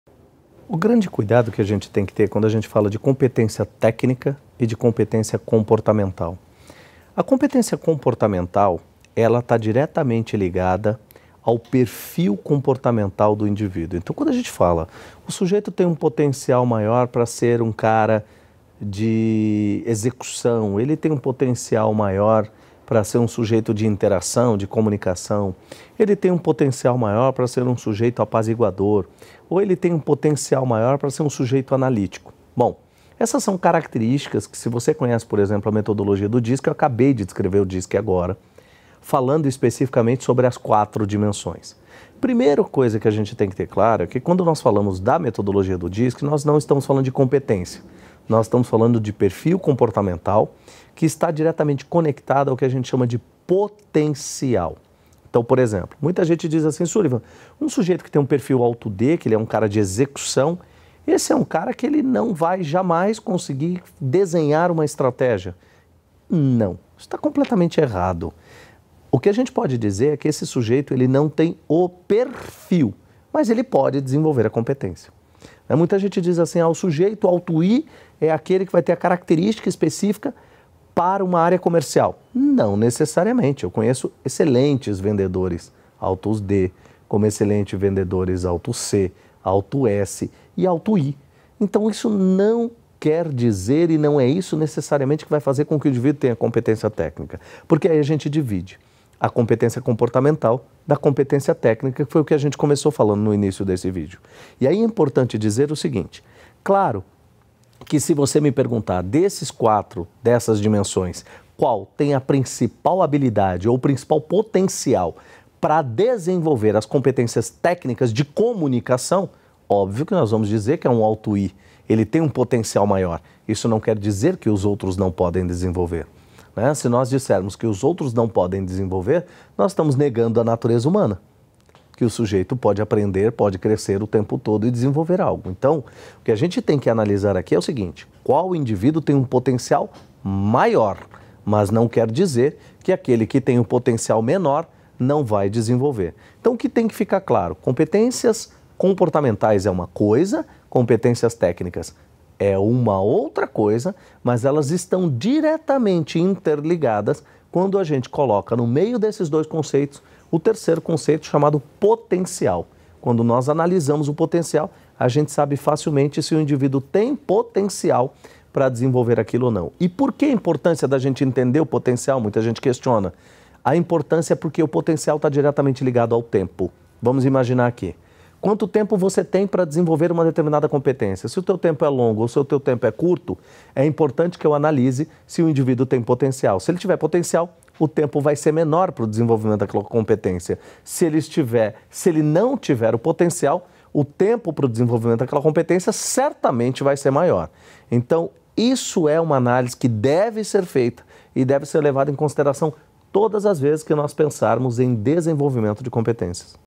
COMPETÊNCIA TÉCNICA E COMPETÊNCIA COMPORTAMENTAL | Sulivan França
Entenda a diferença entre competência técnica e competência comportamental.